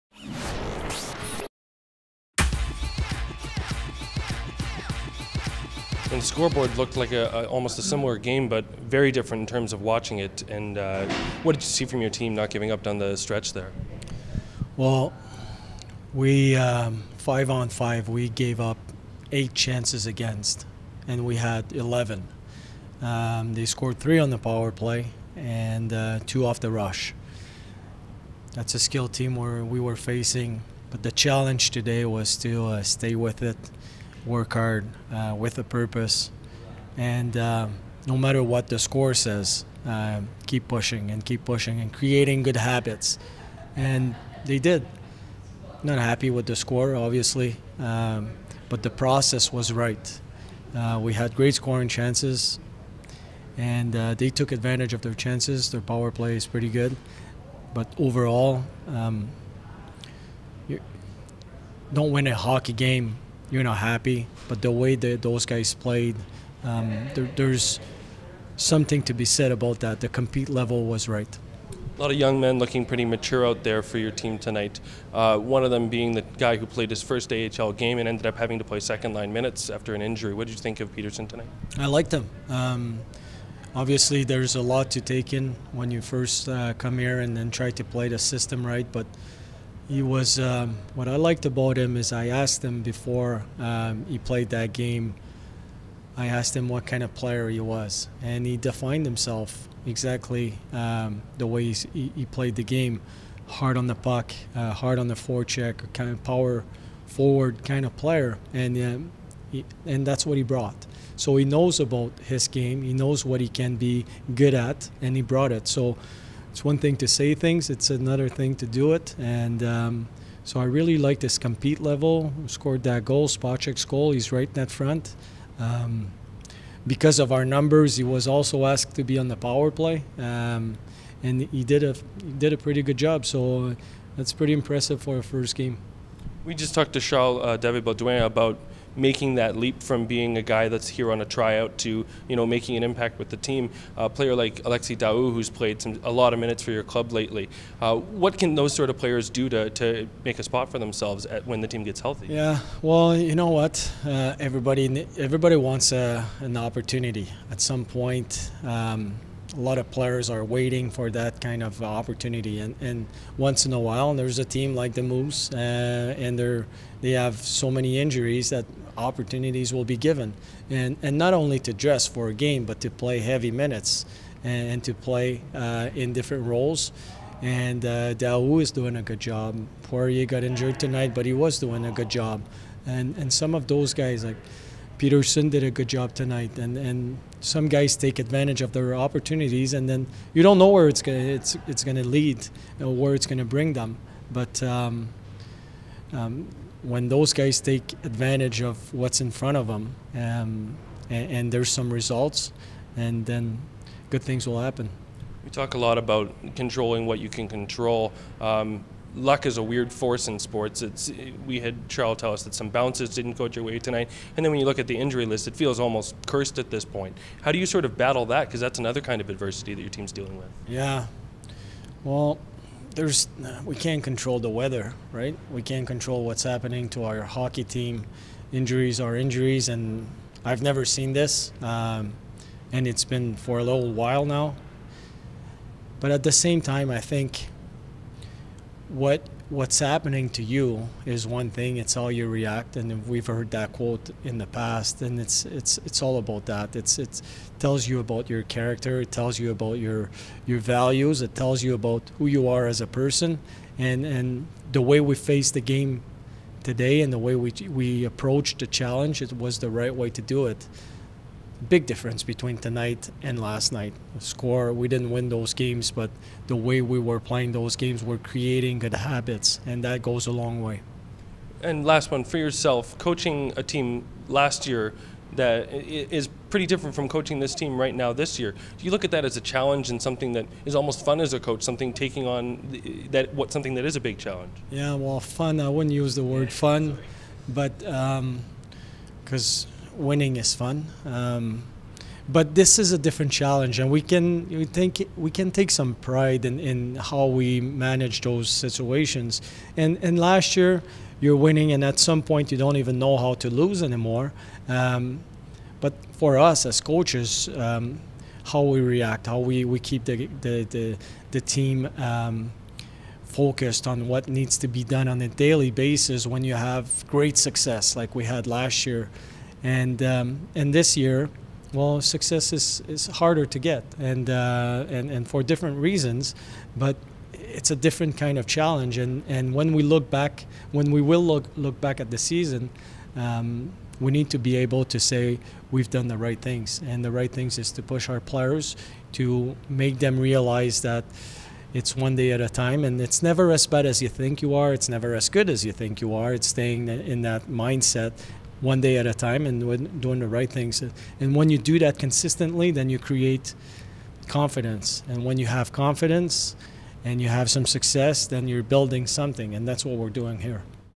POST-GAME | Pascal Vincent
Dec. 15th, 2018 at Bell MTS Place against the Toronto Marlies.